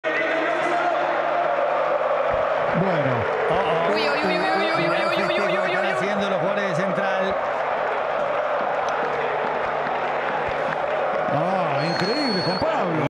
El baño de Fatu Broun a Miguel Ángel Russo tras el título de RosarioCentral
No es sólo una liga, es la liga de los campeones del mundo.